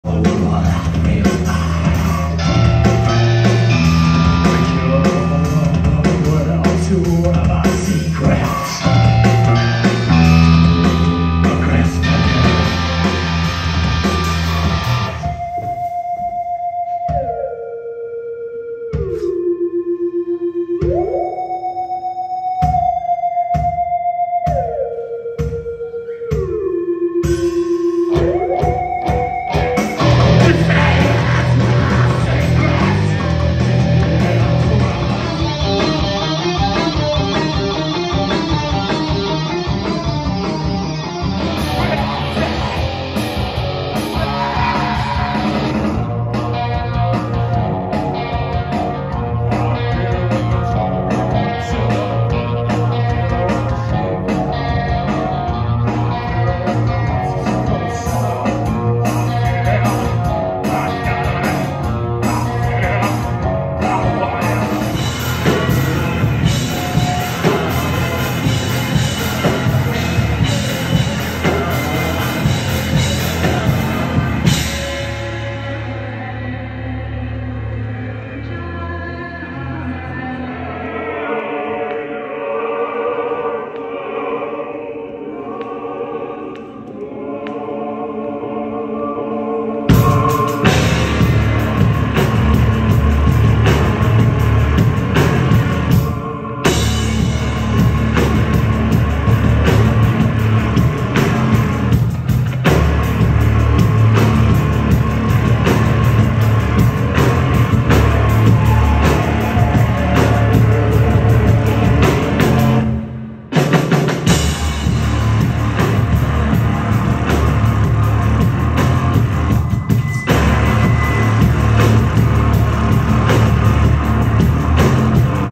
Clips from Metal Monday 7/31/23
Filmed on July 31 at the Metal Monday at the Savage Henry Comedy Club in Eureka, CA. Performers were two local bands, Image Pit which is always a blast to witness, Sarcophilus Satanicus which is another local gem of a band, and a touring band from LA called Thief. Was awesome coming out to this, loved all the bands I saw.

Performers were:
Image Pit
Sarcophilus Satanicus
Thief